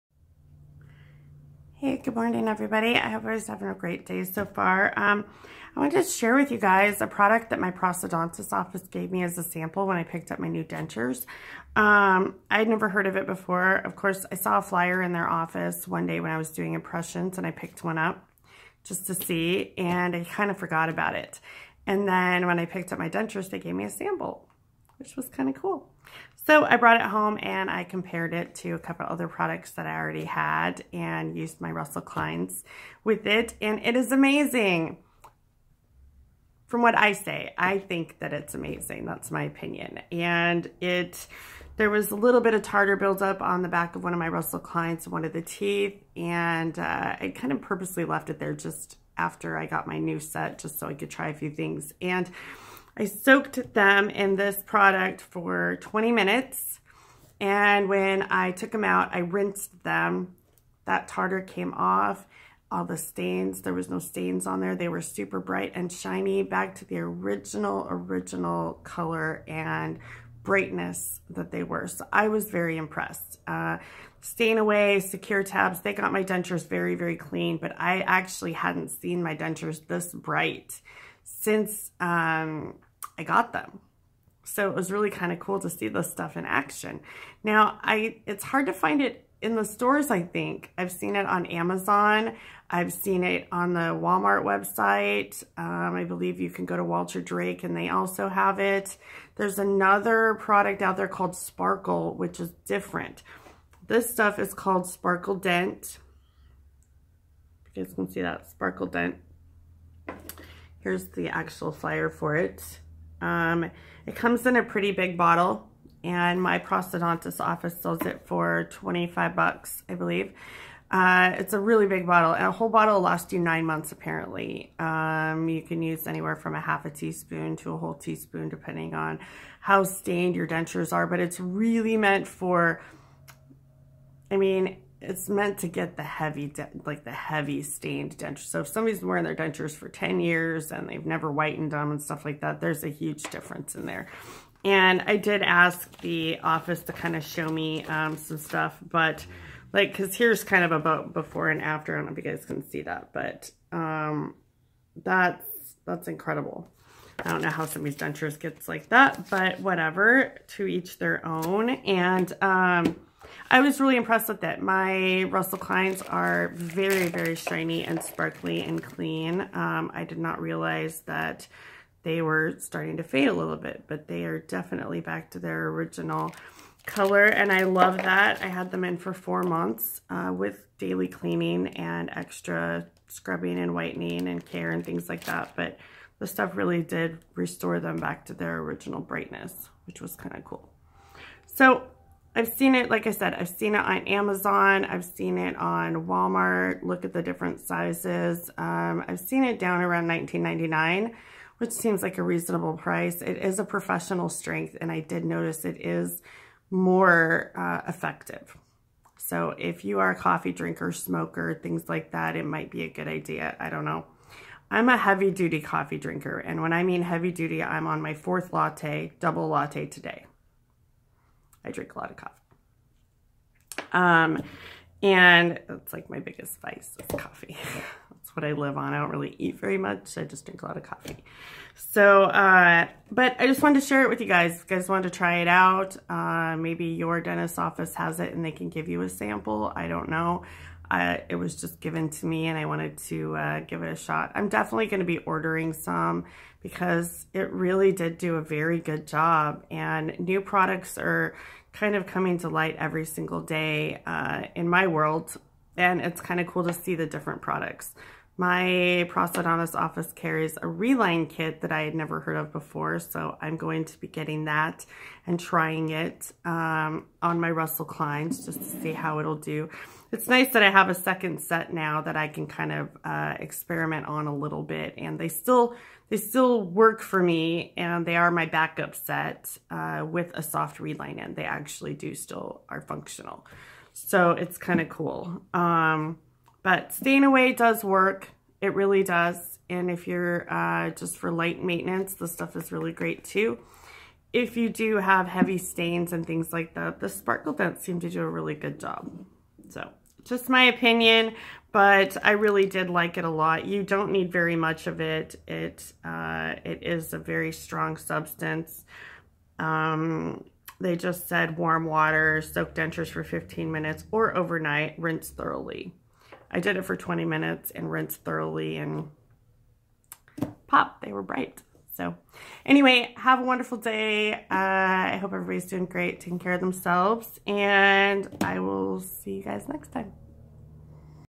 Professional denture whitener/cleaner review
Sample product that made my dentures shine! Returned them to the original brightness. Sparkle Dent.